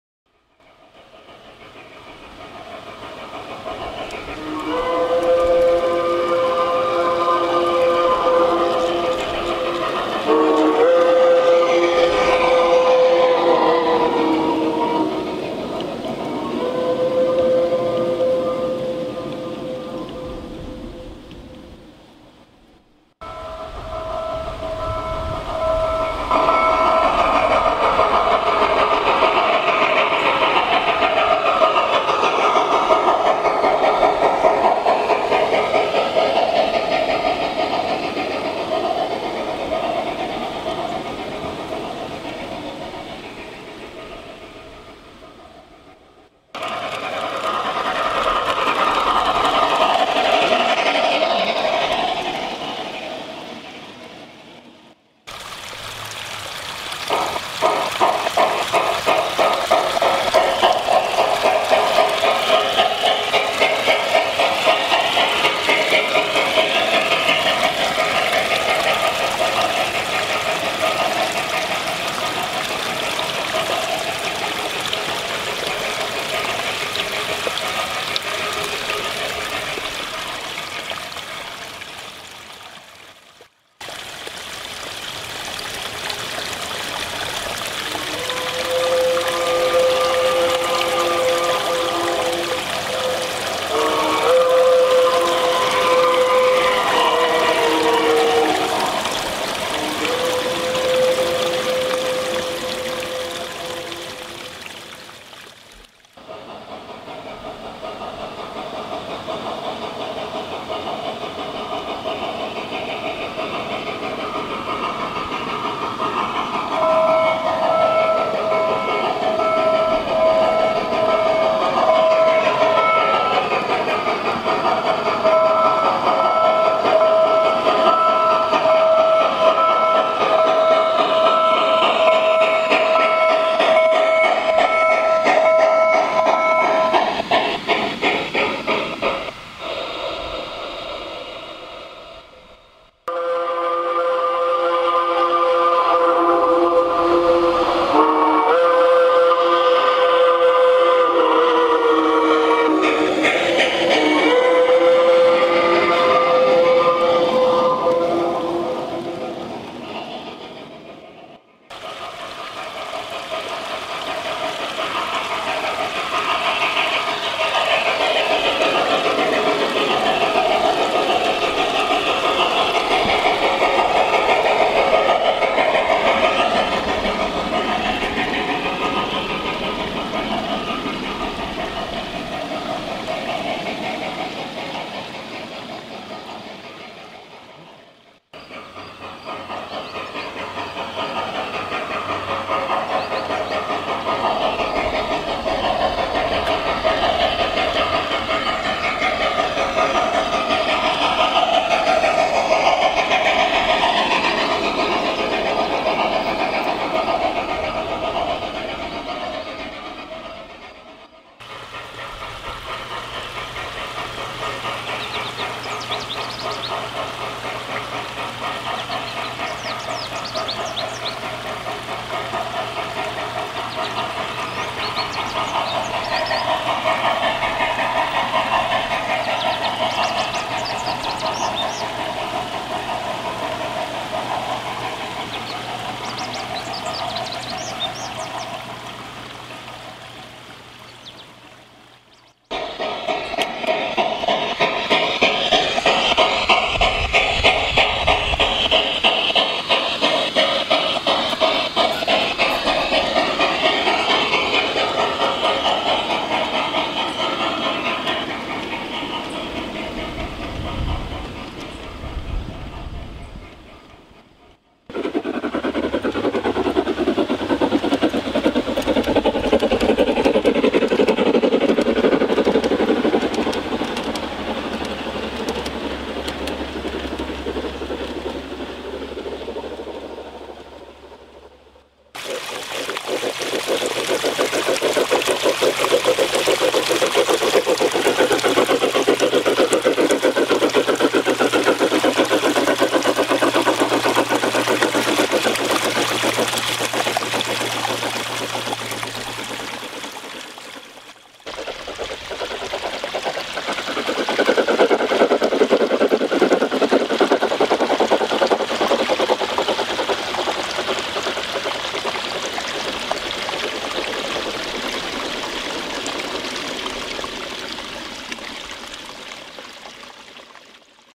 The Kettle Valley Railway in G scale !
This is one amazingly well done G scale railway based on the Kettle Valley Railway (KVR)   Thanks very much to Brian the owner of this layout for helping me get this footage of his layout ! Please Enjoy !